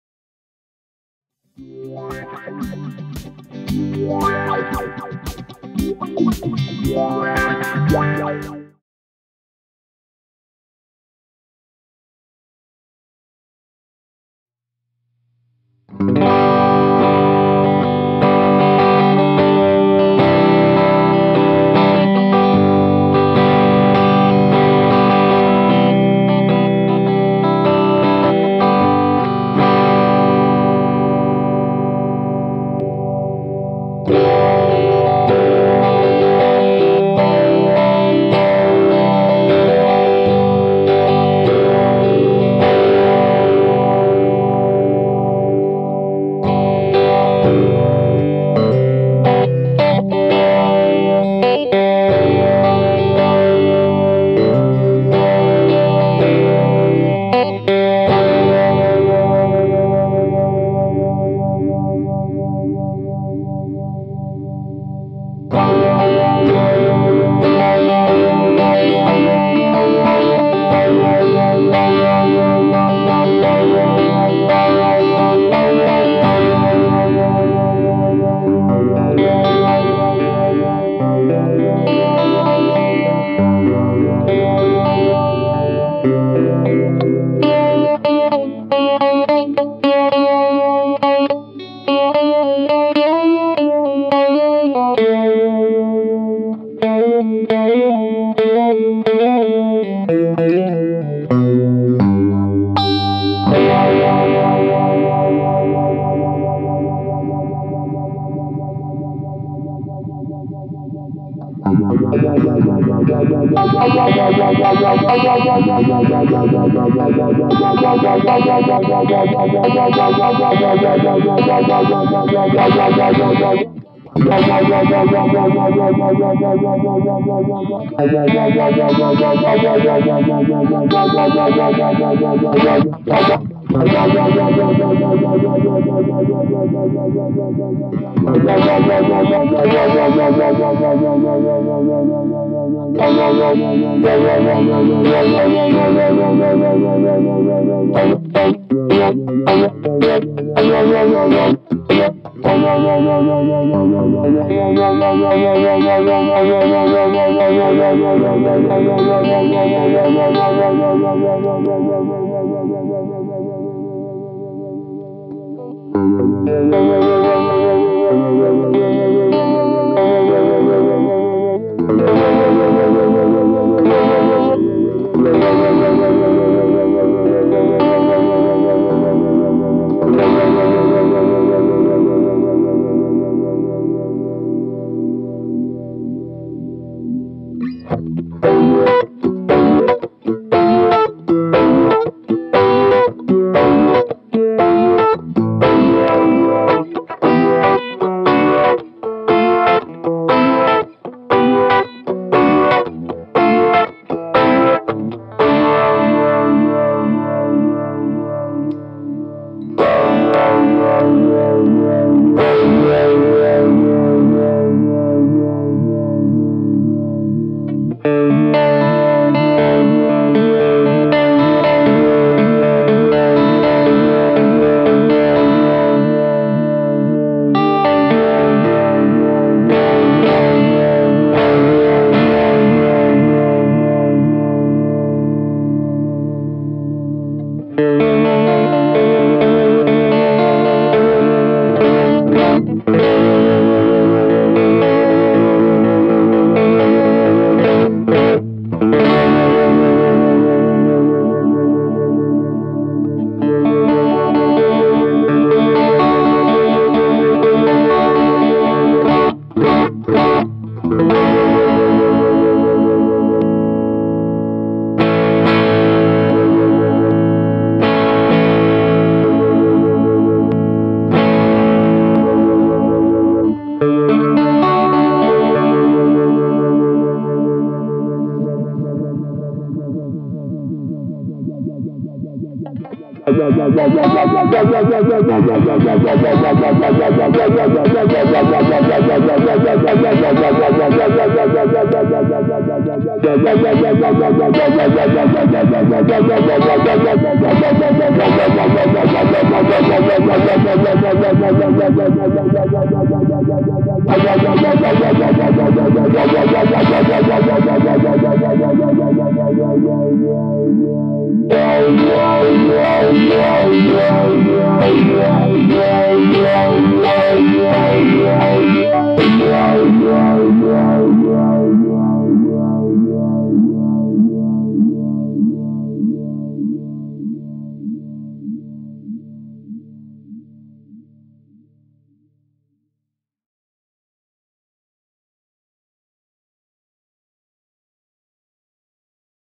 Walrus Audio Lillian Multi-Stage Analog Phaser
This is a demo video of the Lillian Multi-Stage Analog Phaser effects pedal from Walrus Audio performed and produced by Pedal of the Day. for more effects pedal reviews and demos, and be sure to check out more of the videos on our YouTube channel as well. Cheers!

Tone Tubby Purple Haze

sE Electronics & Shure Microphones

Spectraflex Cables